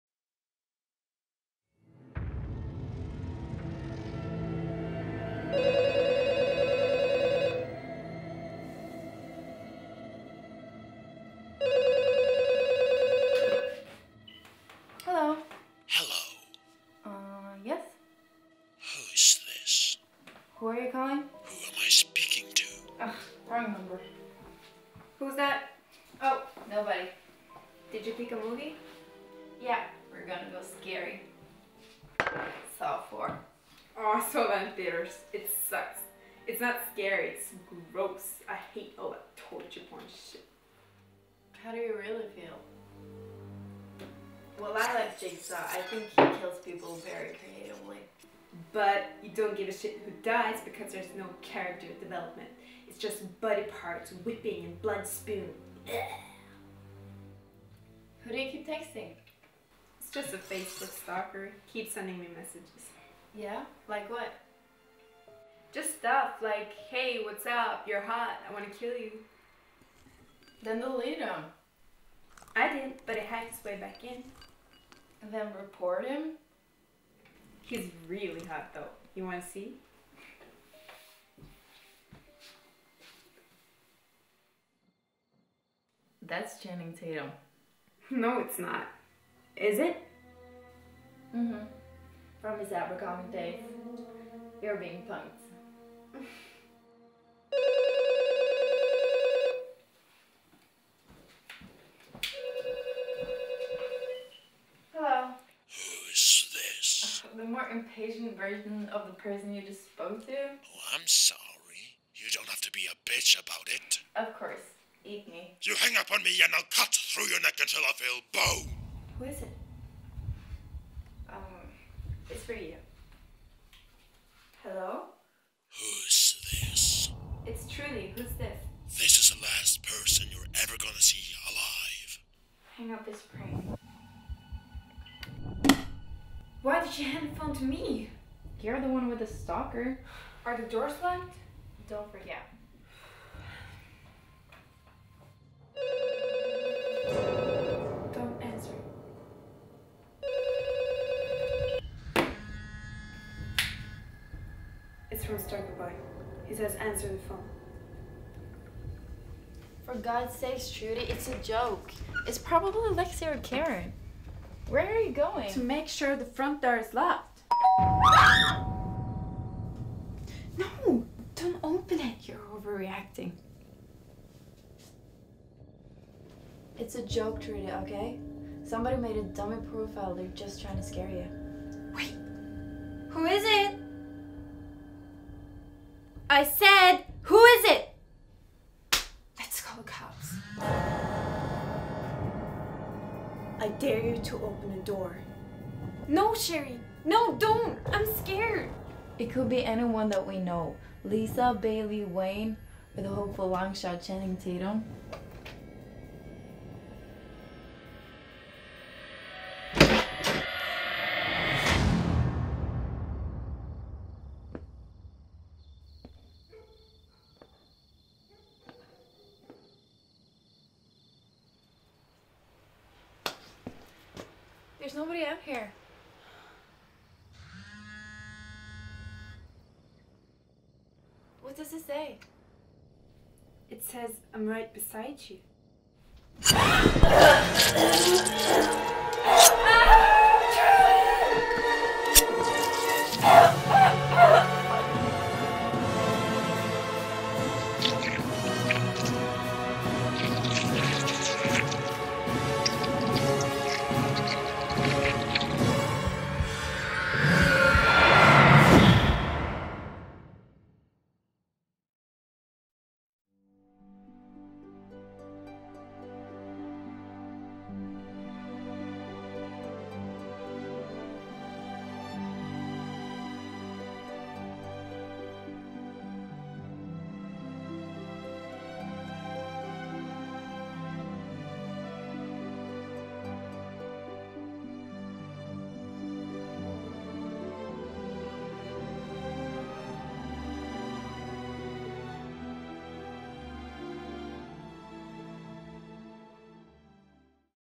Scream 4 - School Project (to recreate a scene)
Filmed and edited by Marius Solberg